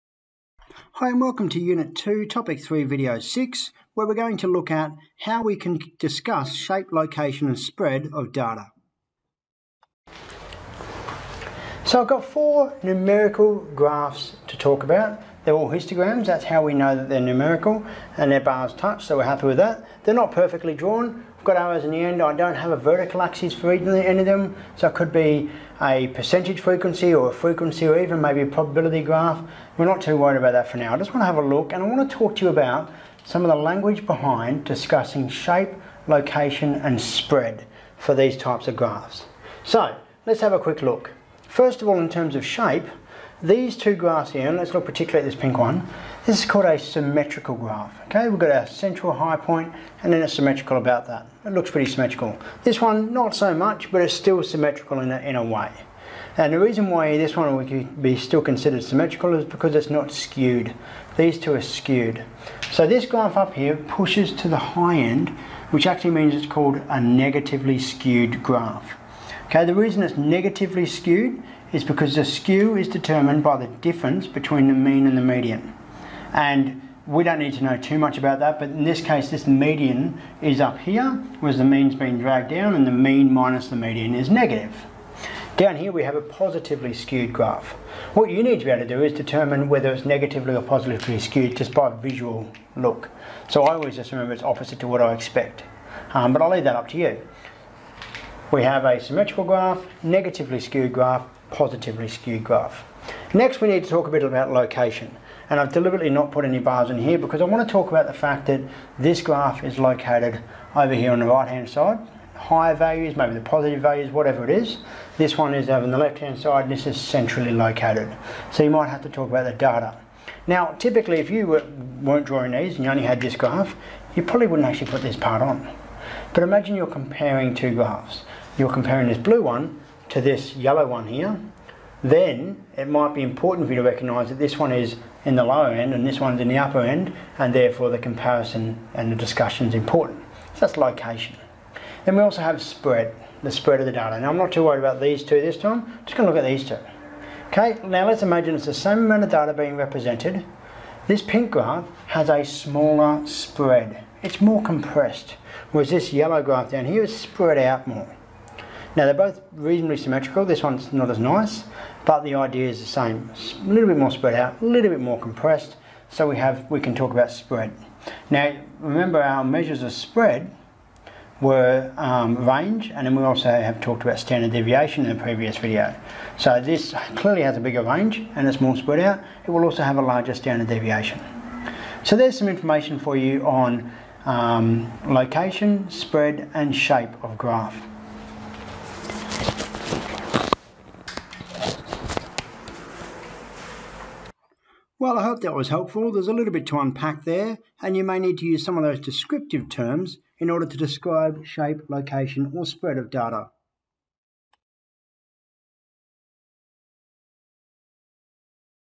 General Mathematics - U2T3V6 - Describing shape, location and spread.
In this video we use mathematical terms and evidence ot describe the shape, location and spread of a set of data.